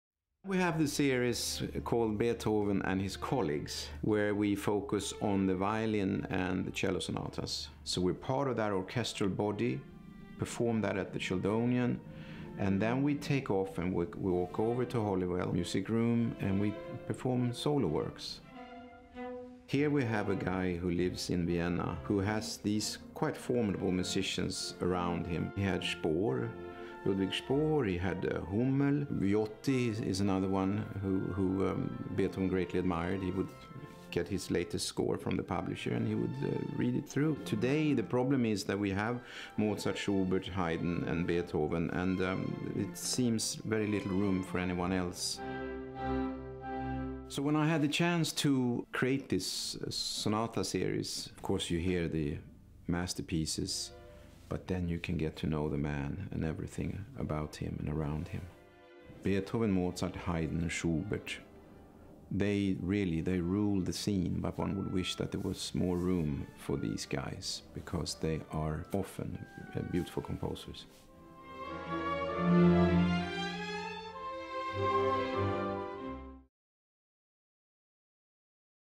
OPO: Beethoven & his Colleagues Recital Series
As part of our Oxford Beethoven Festival, we are presenting six chamber music recitals at the Holywell Music Room.

These concerts in March and November 2020 will explore the works of Beethoven and his contemporaries.